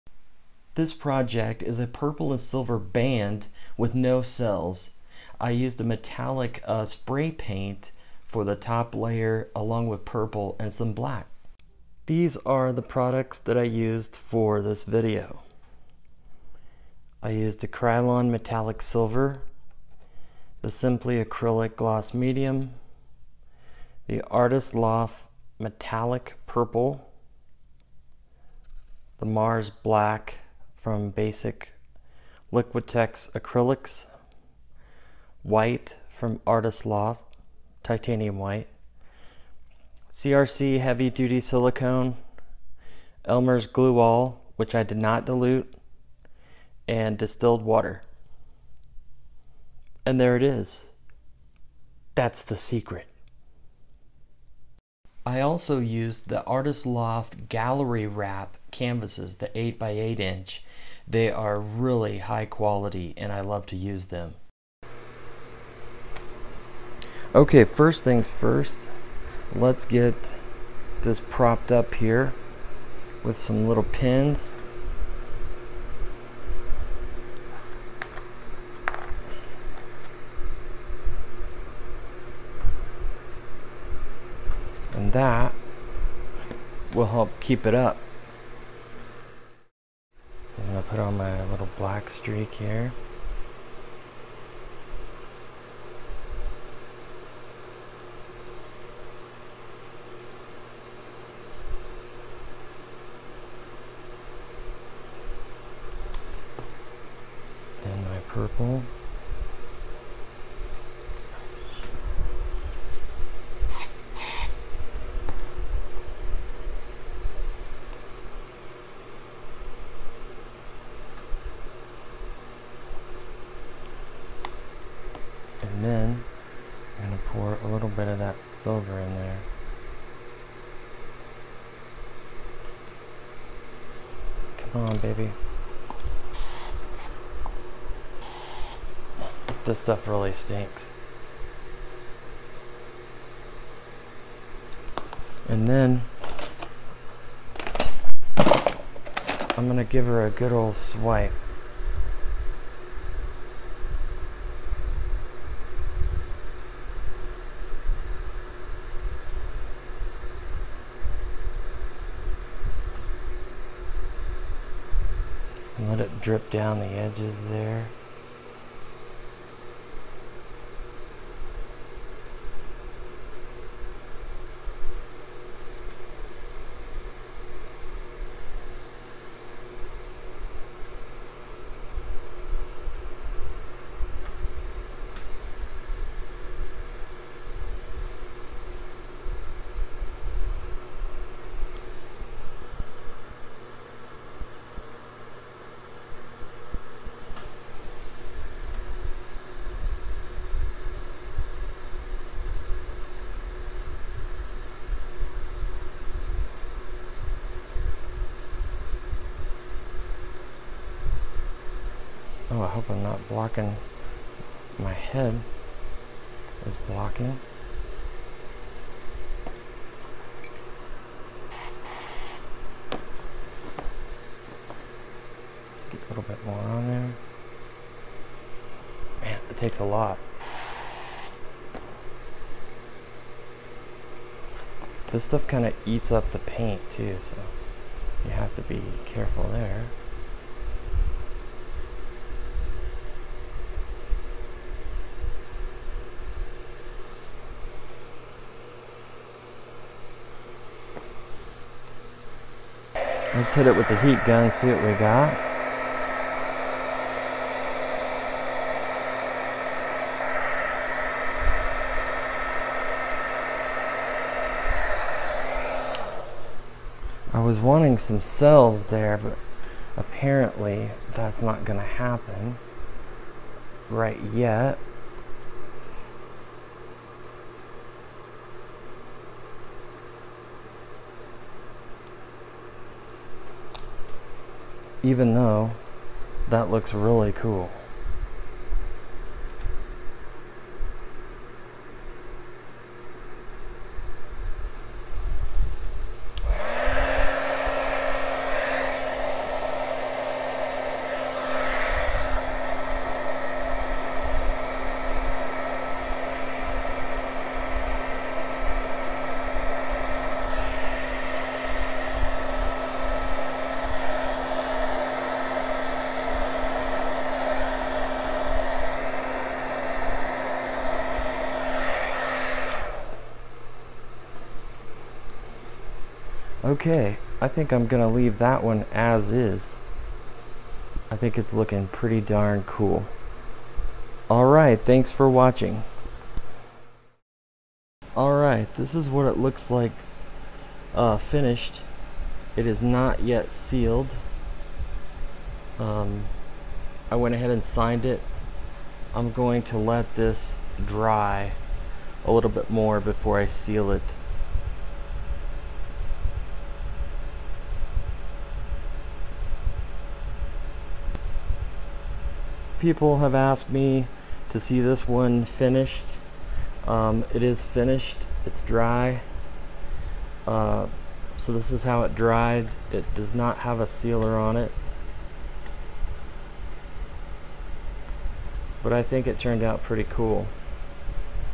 Purple Band Pour So Simple!
In this tutorial I did a purple stripe or band poured across an 8x8 Artist's Loft canvas.

Fluid pour with acrylic paint, PVA or Elmer's Glue, Acrylic Gloss, Acrylic Paint, Metallic Paint and Silicone mixed together. This technique did not turn out any cells, but it came out really nice, regardless. I used a technique of what's called "negative space" and left white space out in the open to give a nice clean effect.

If I can do it... You can do it. It's pretty simple and most of all it's fun.